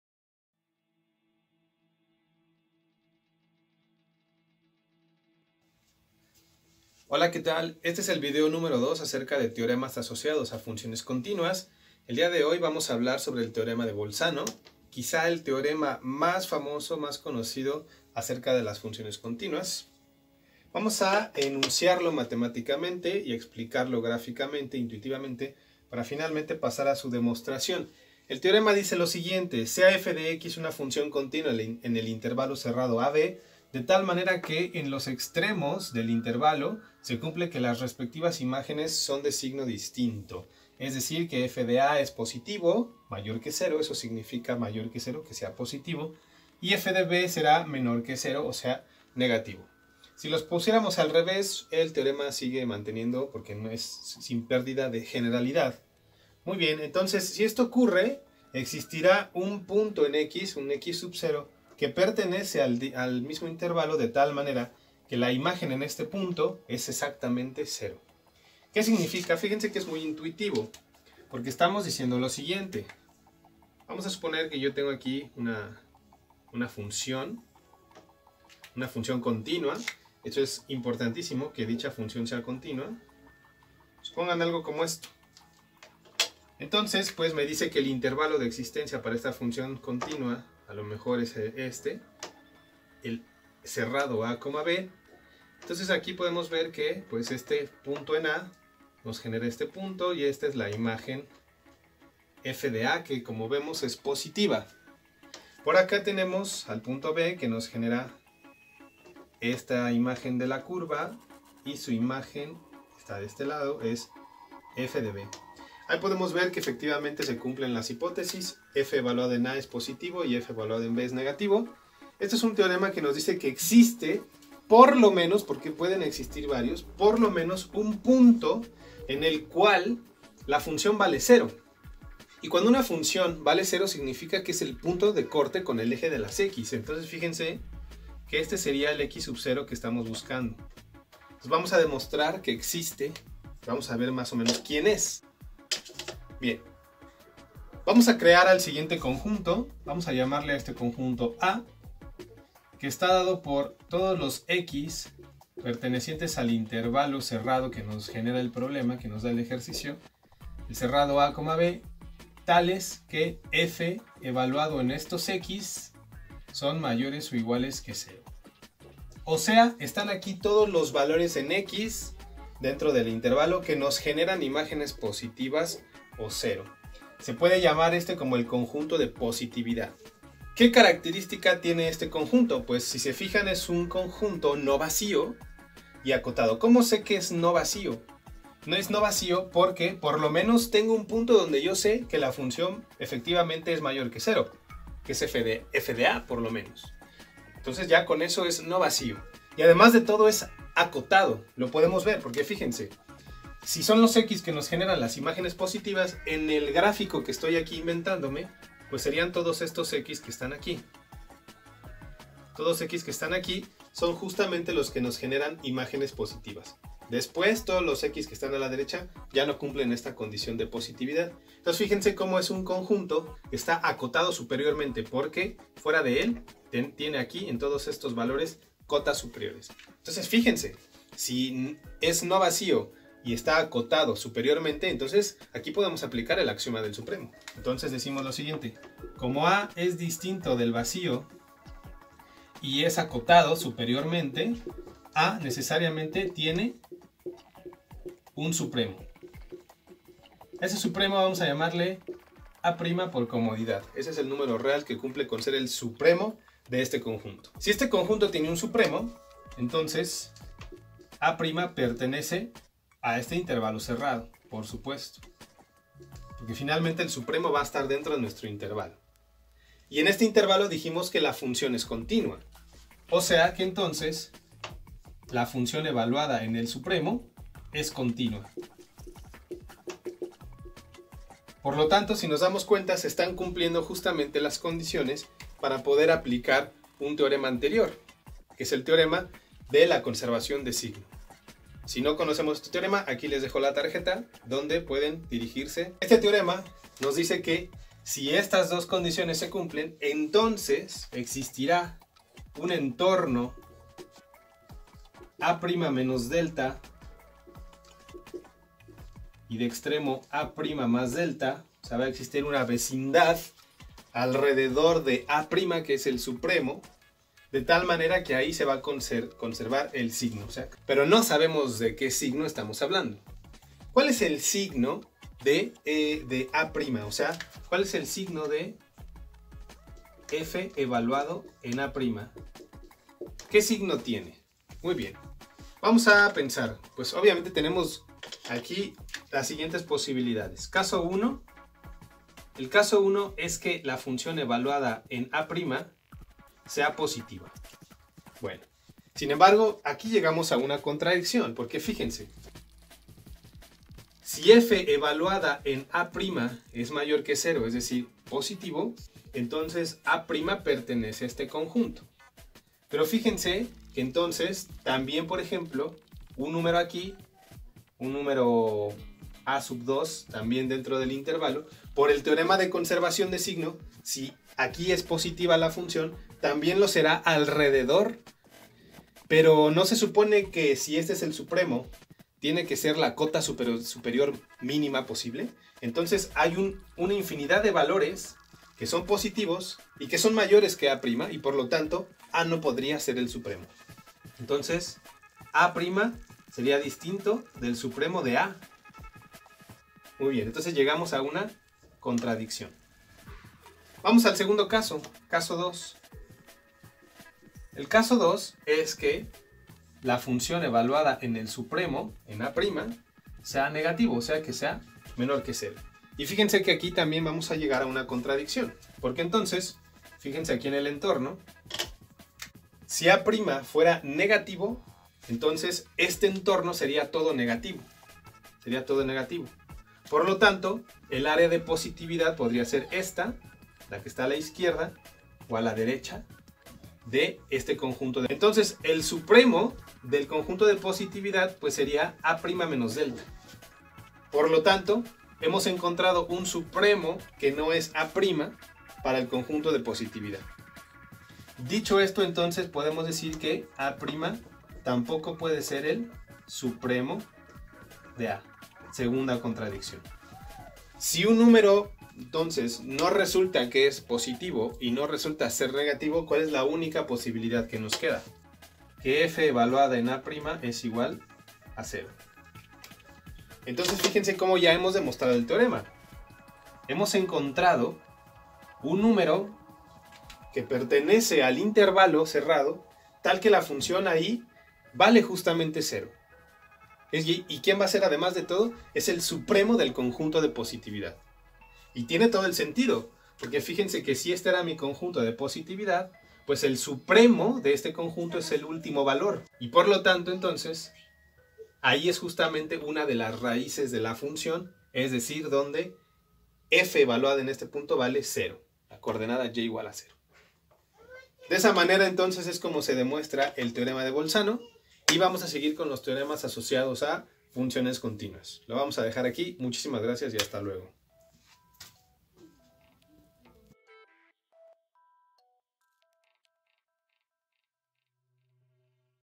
FUNCIONES CONTÍNUAS II: teorema de Bolzano (Demostración)
En este video demostramos còmo mediante el axioma del supremo y el teorema de conservaciòn de signo, podemos demostrar el teorema de Bolzano.

Para ver la lista de reproducción completa sobre teoremas asociados a funciones contínuas, haz click en el siguiente link